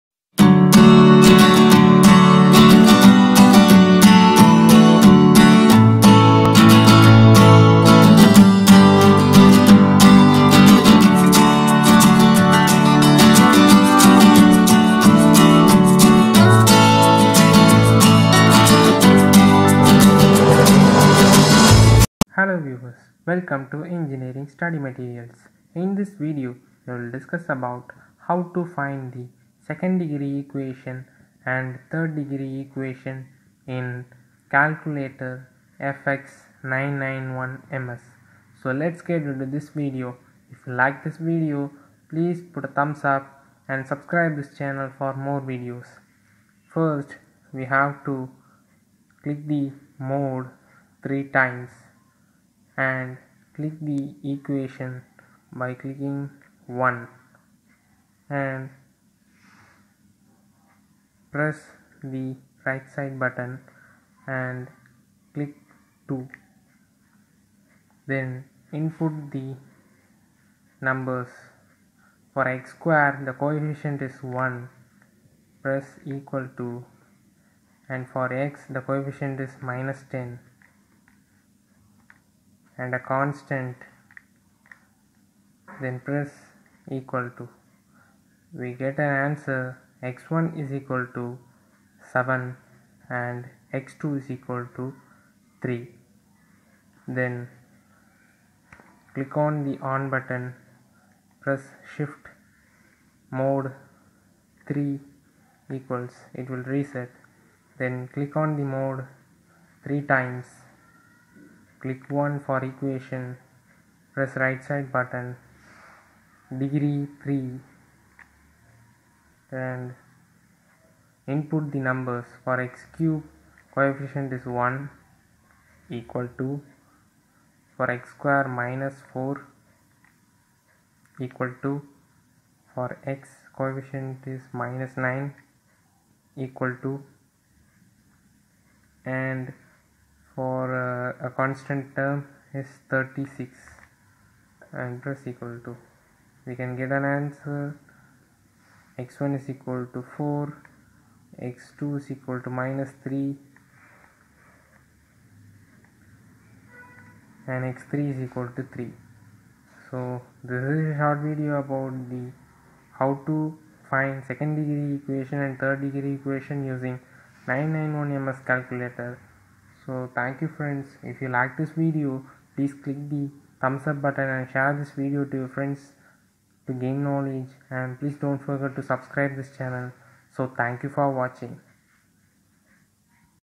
How to Solve Quadratic and Cubic Equations using Scientific Calculator CASIO fx 991 MS
Thank You Viewers For 1,30,000 Views for the Channel.

In this video I will teach you How to Solve Quadratic and Cubic Equations using Scientific Calculator CASIO fx 991 MS .

In  CASIO fx 991 MS we can do lot of things such as
Logarithm,
Trignometry
Matrix
Standard Deviation ,etc